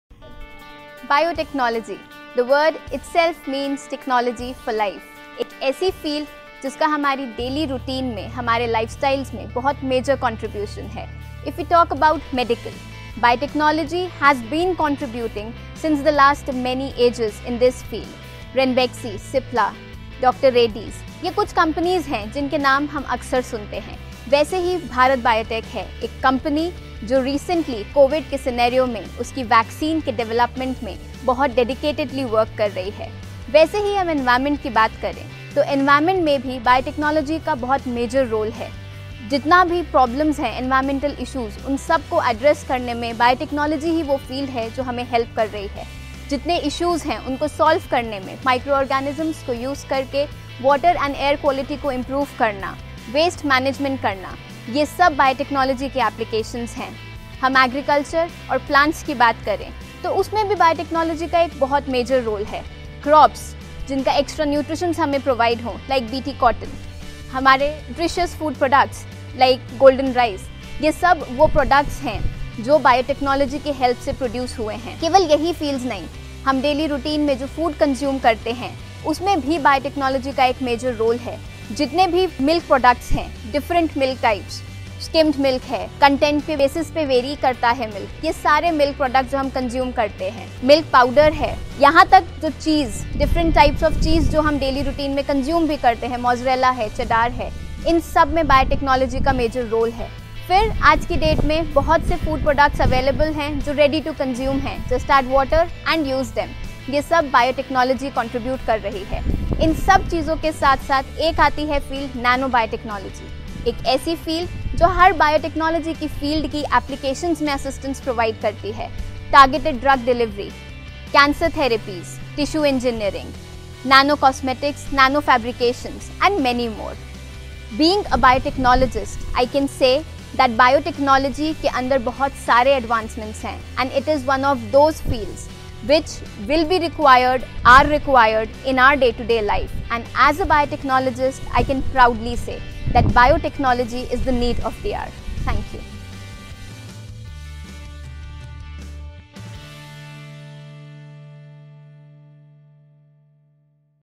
Amazing Advancements in Biotechnology | Dr. B. Lal Institute of Biotechnology
"Biotechnology" the term sounds so bookish!

But, if you wear a naturalistic lens, you will surely find the term in each and every advancement surrounding you.

So, let this video be the lens through which you can peep into various advancements in Biotechnology to know its prevalence outside those books.

Otherwise, you may not receive a notification when we upload a video.